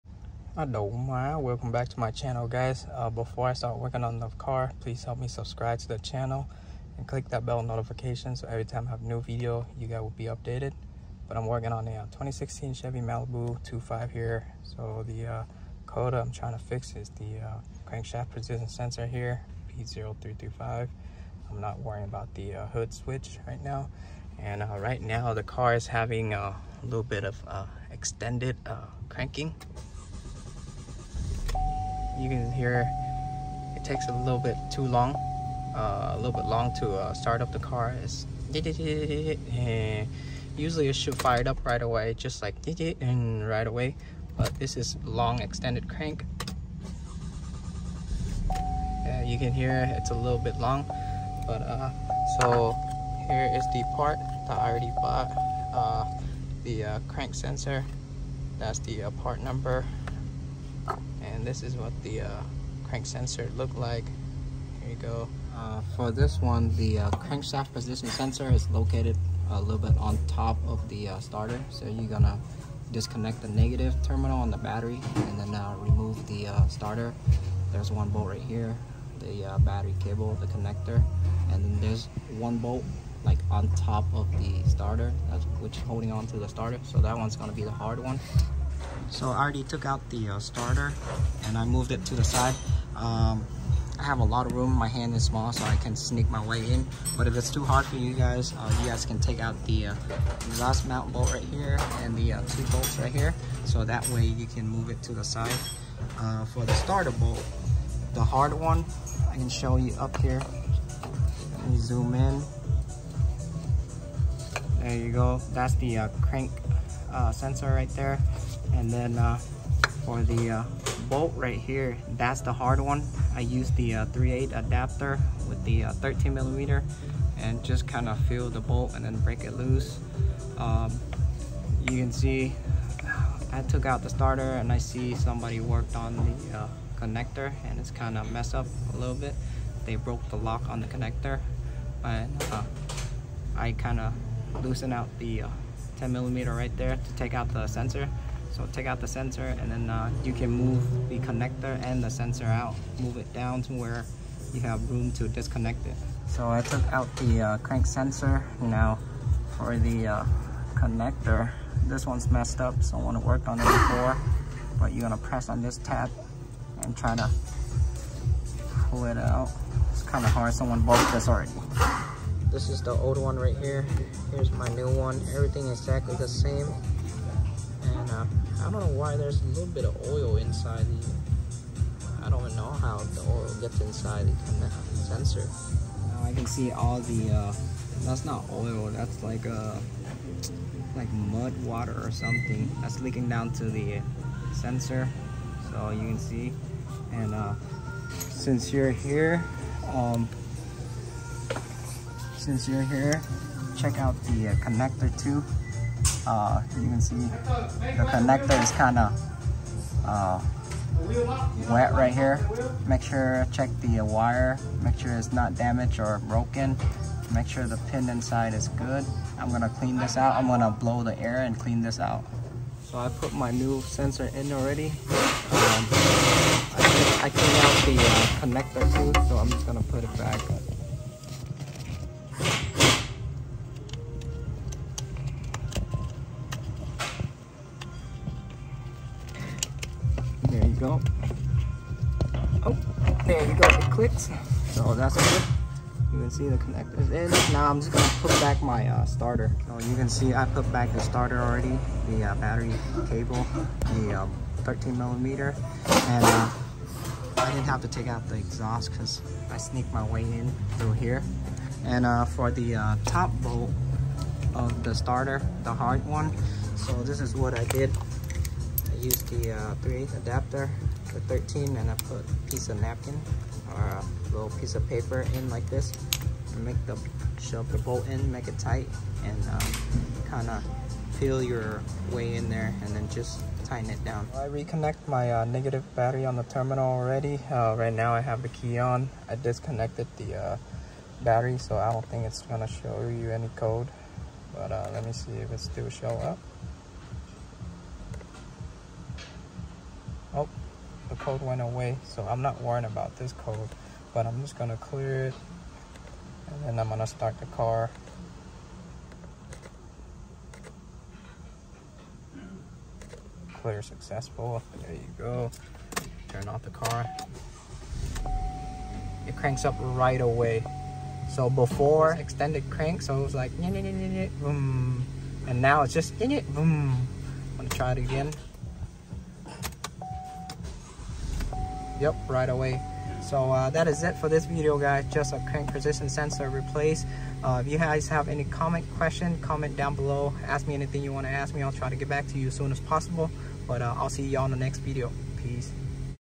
2008 To 2016 Chevy Malibu Crank Position Sensor Replace - Code P0335
Chevy Malibu crank position sensor replace. Long extended cranking - code p0335.